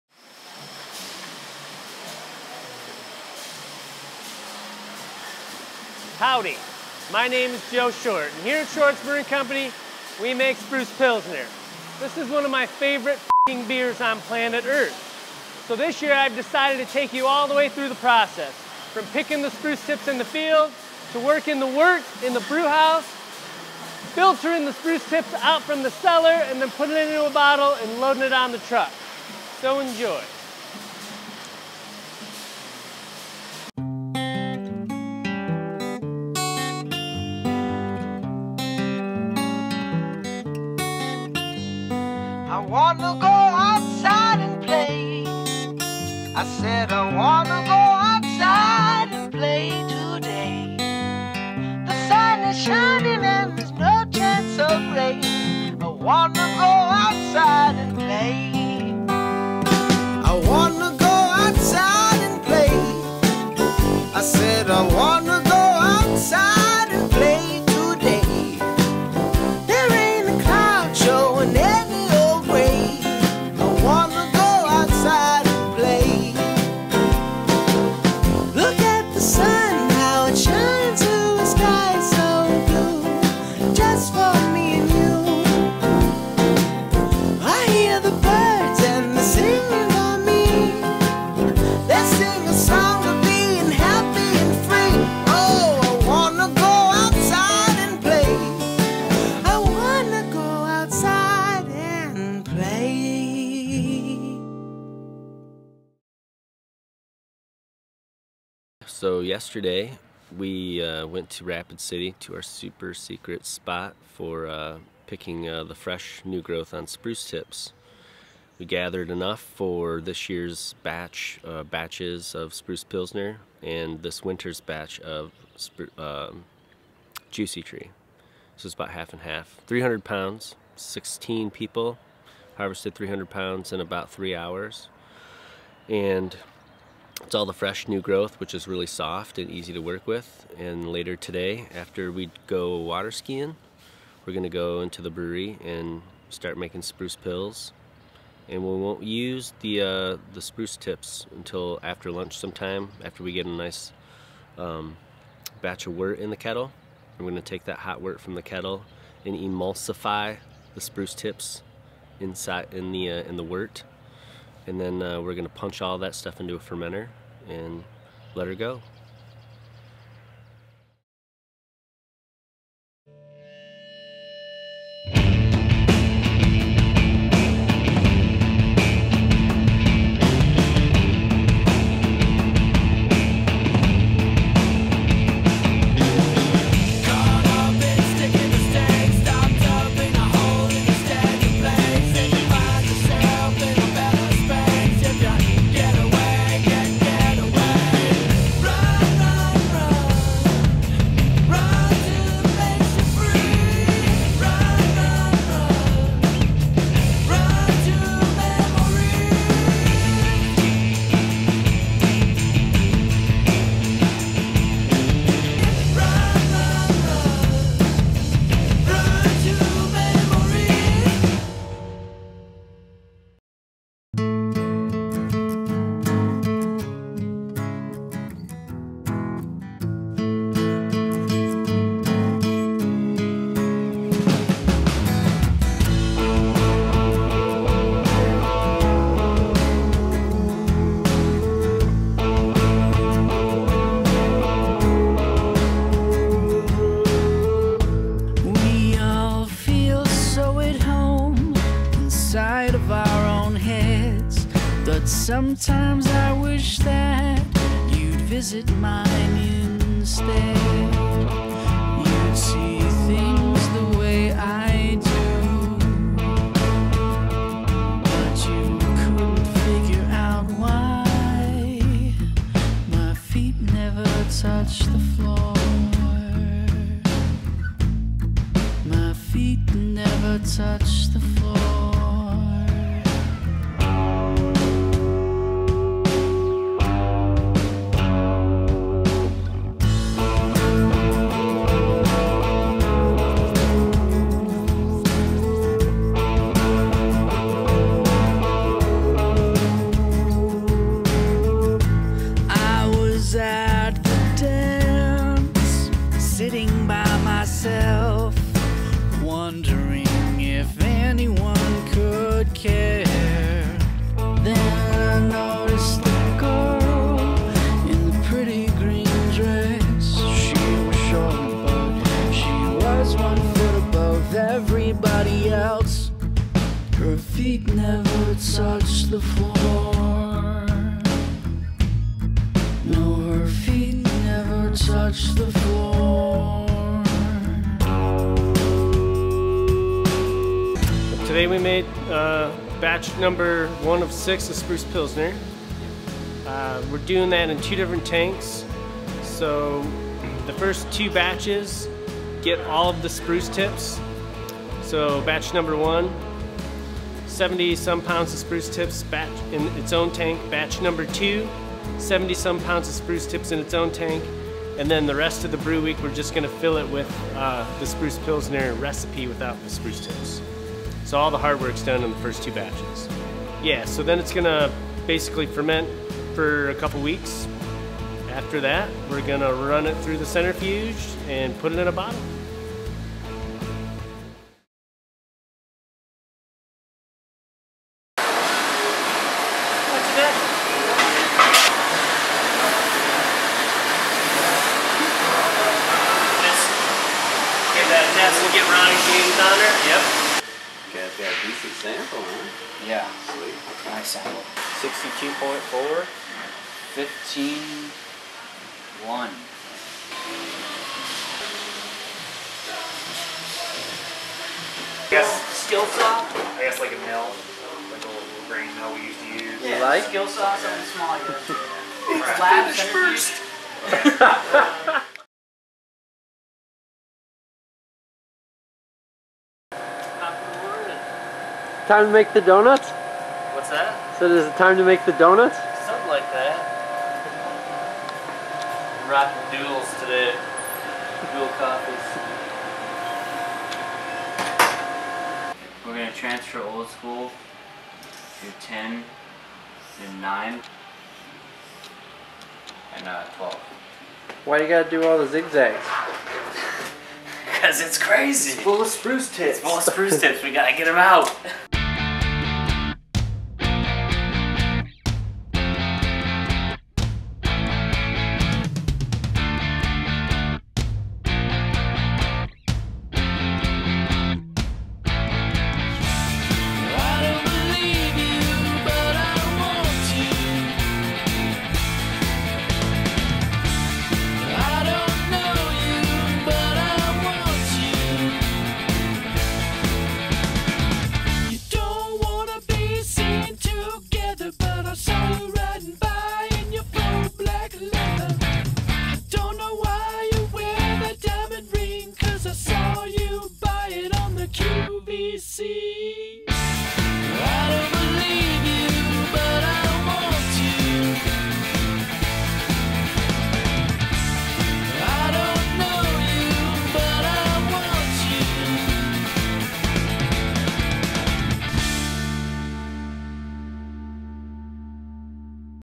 Spruce Pilsner From Pick to Package
In this video Joe Short takes you through the entire process of what it takes to make Spruce Pilsner.